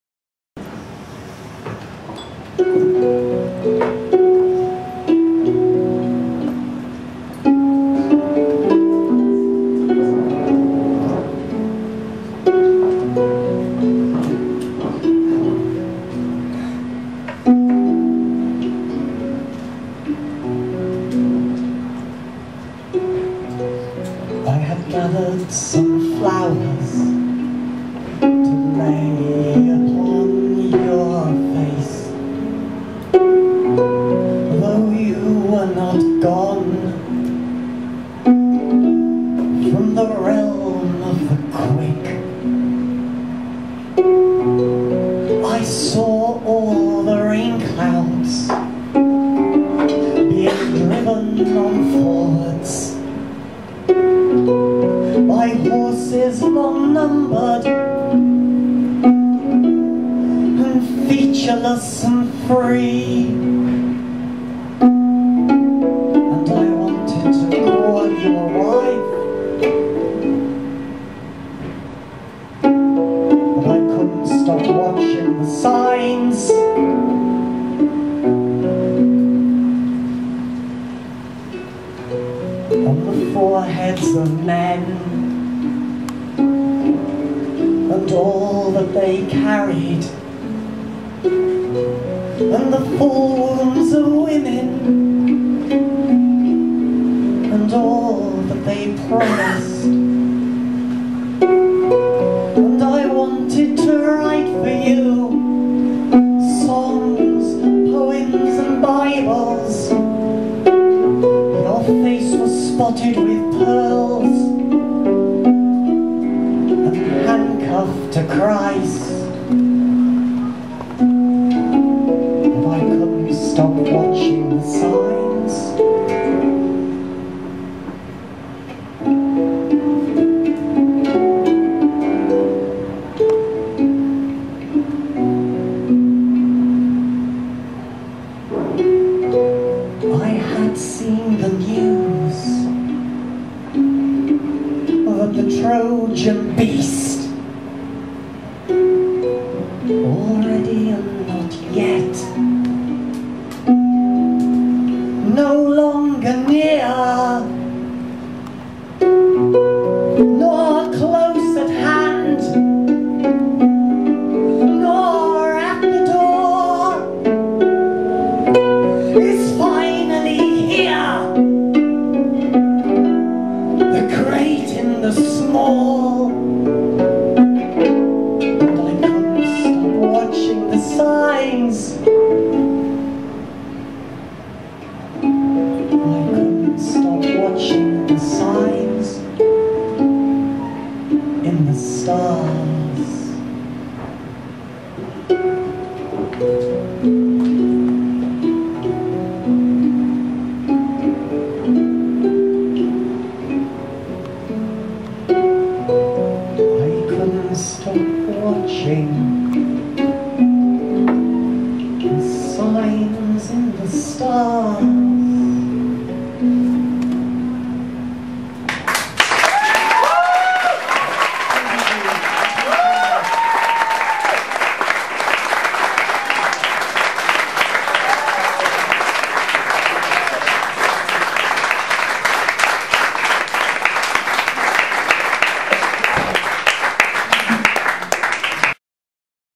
Baby Dee and David Tibet live at the Bistroteque
Baby dee and David Tibet performing at Londons Bistroteque, Sept 06